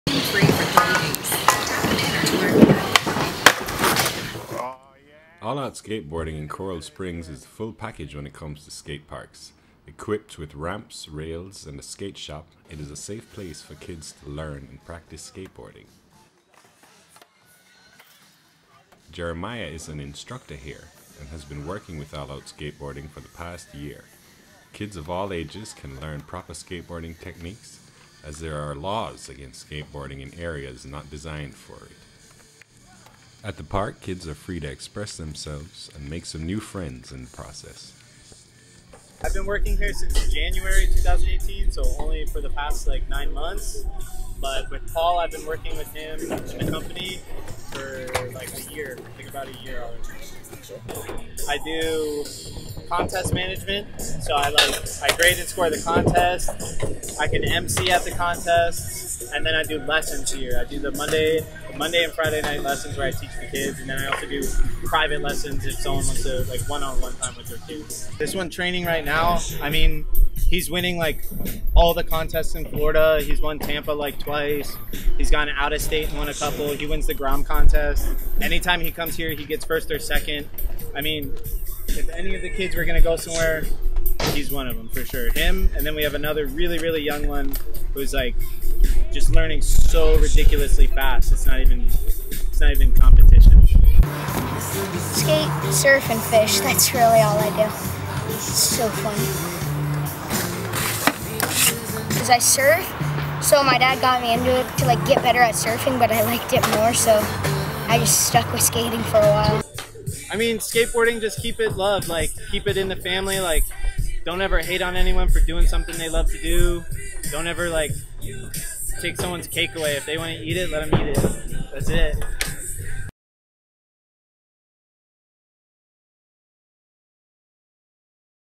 All Out Skateboarding Park In Coral Springs ft Jeremiah St. Louis
All Out Skateboarding Park is a full service skateboarding park equipped with ramps, rails and a skate shop. Kids of all ages can enjoy the park and compete in some healthy competition.